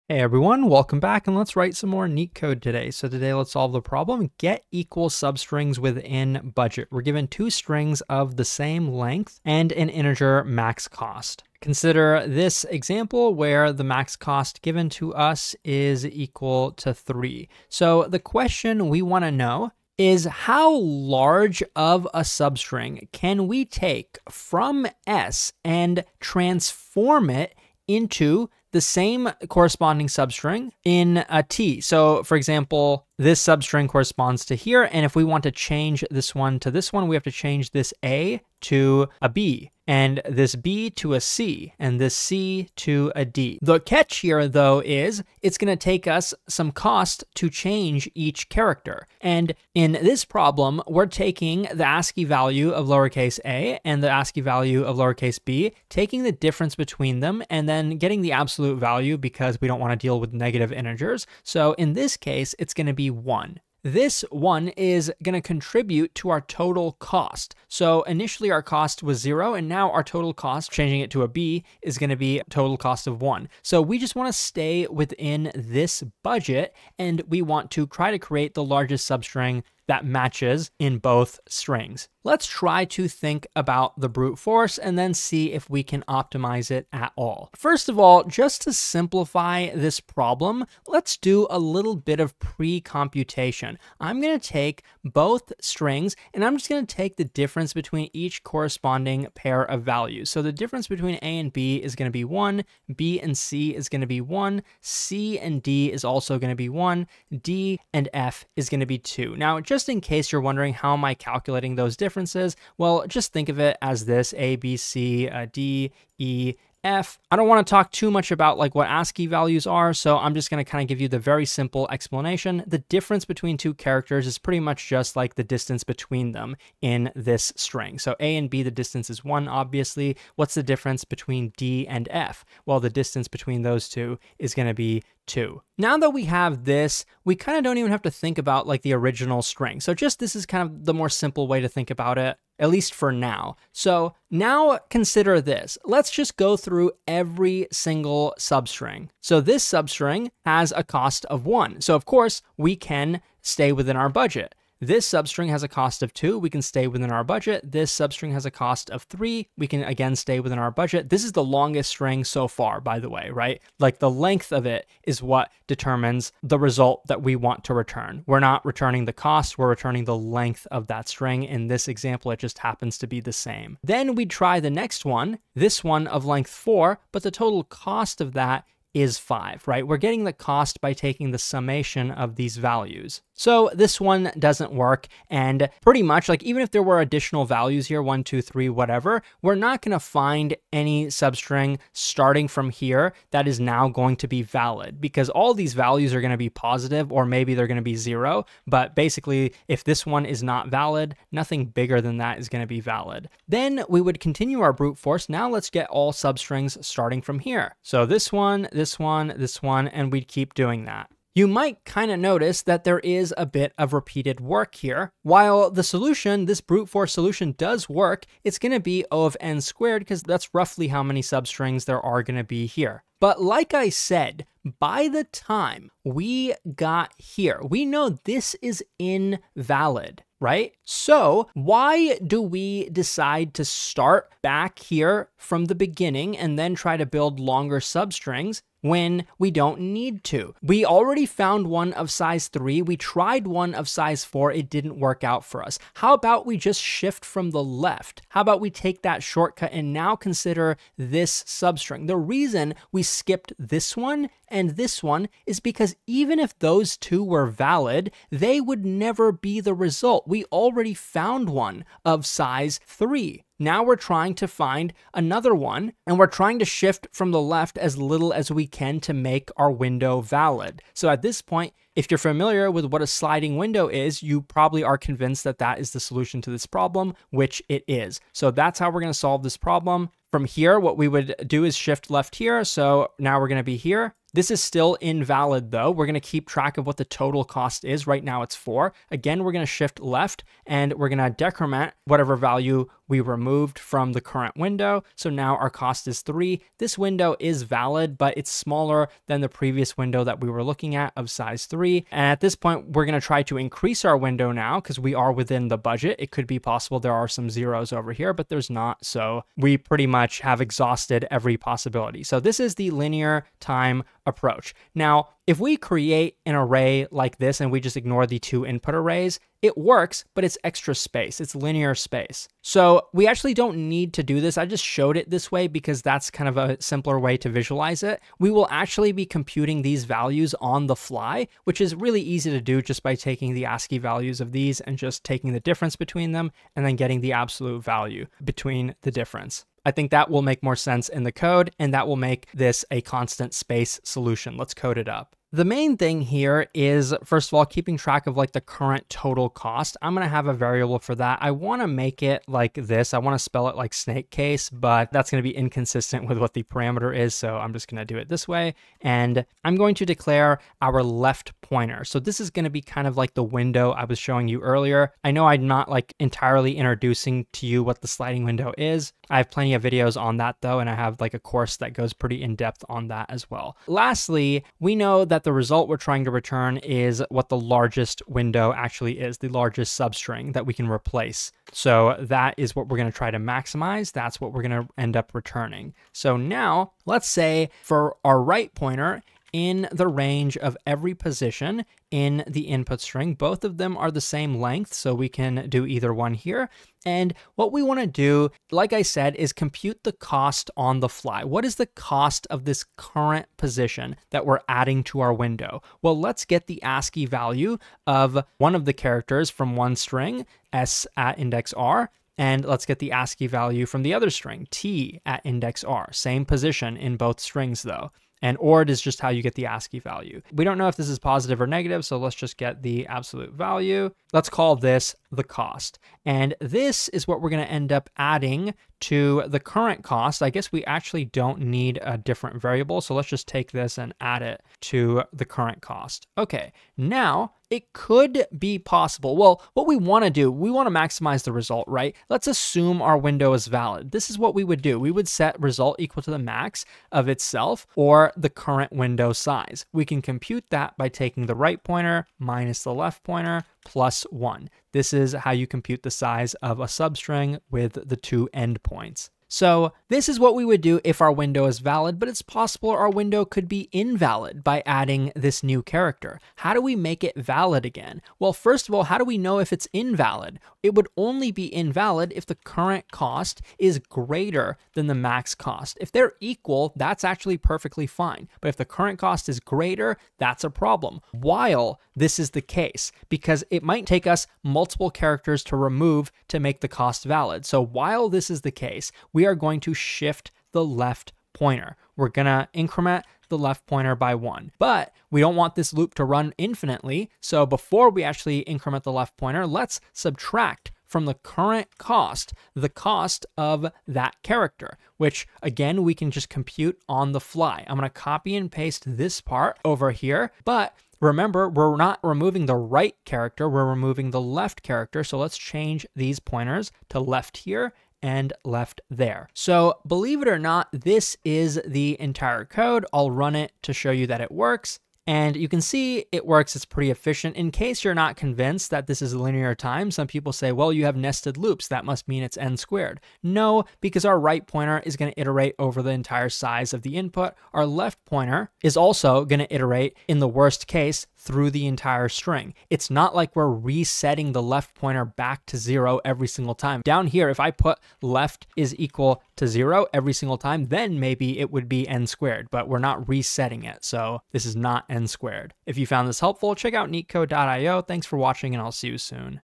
Get Equal Substrings Within Budget - Leetcode 1208 - Python
0:00 - Read the problem
0:30 - Drawing Explanation
5:45 - Coding Explanation

leetcode 1208